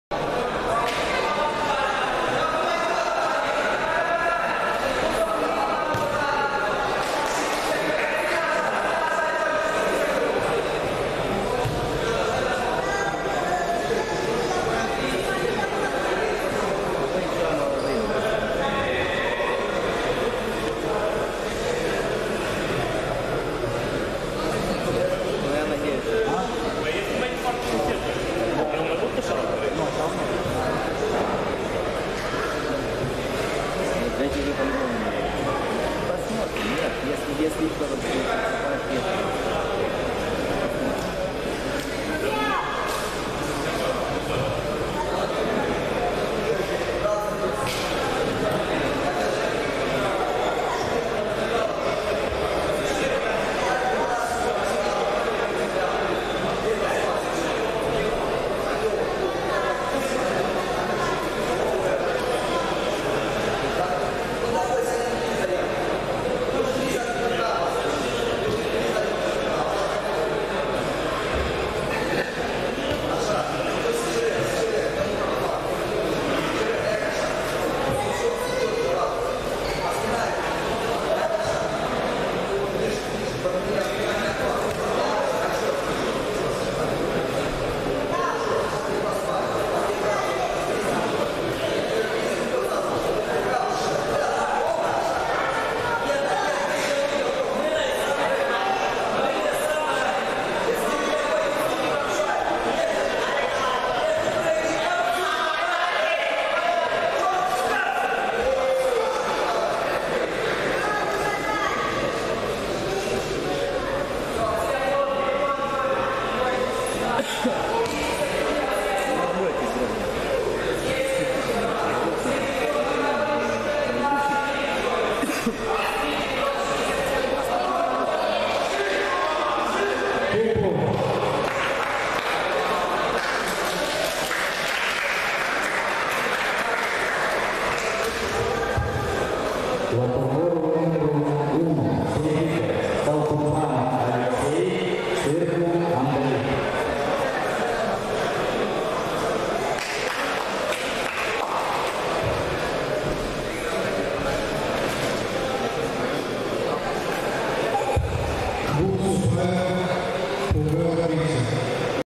66 ПОЕДИНОК ADCC MD 2022 Campionatul Open Moldova 09 04 2022
Редактировать
ADCC MD 2022 Campionatul Open Moldova (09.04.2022)
ПОЕДИНКИ С ПЕРВЕНСТВА
Раздел Lupte.MD на сайте (Judo.MD)